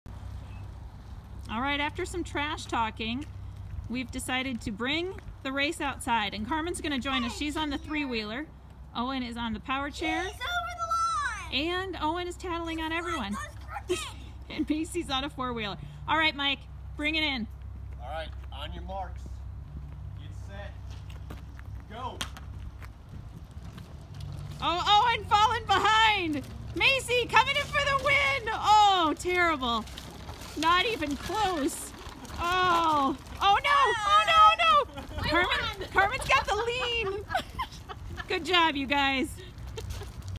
On your marks...Get set...GO!!!!
There's a lot of speculation (read:  trash talking) about whose scooter or chair is the best and the fastest.  We head outside the Greeley show room for some friendly competition (and whining) to determine that winning model.